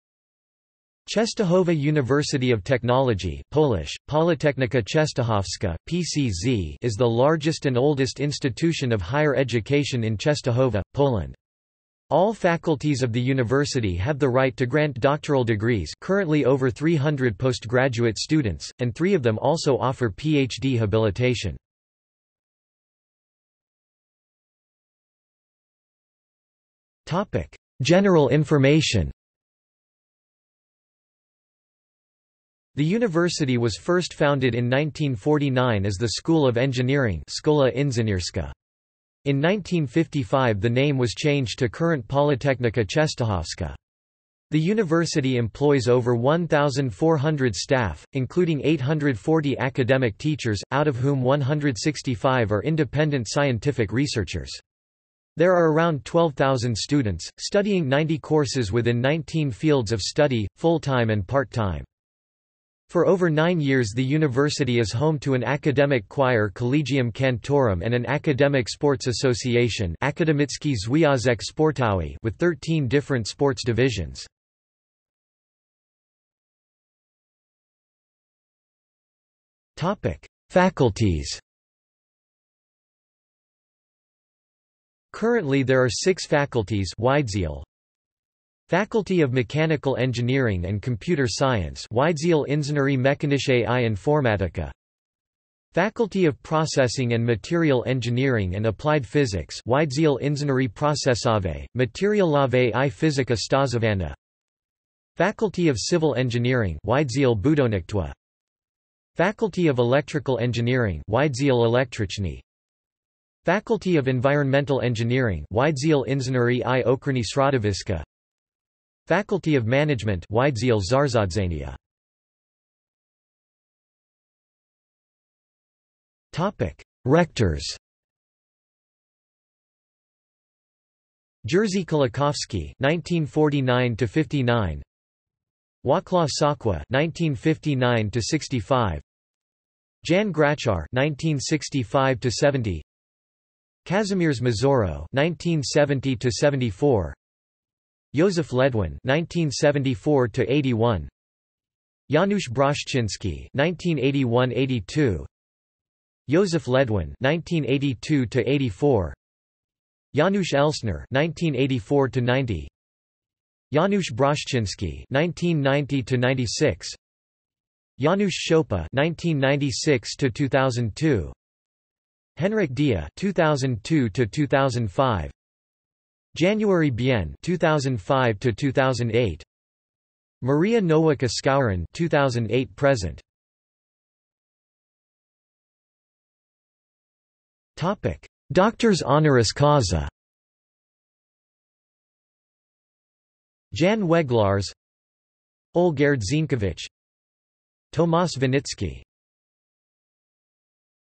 Częstochowa University of Technology | Wikipedia audio article
00:00:23 1 General information
00:01:16 2 Faculties
00:02:02 3 Rectors
00:03:04 4 Doctors Honoris causa

- Socrates

SUMMARY
Częstochowa University of Technology (Polish: Politechnika Częstochowska, PCz) is the largest and oldest institution of higher education in Częstochowa, Poland. All faculties of the University have the right to grant doctoral degrees (currently over 300 post-graduate students), and three of them also offer PhD habilitation.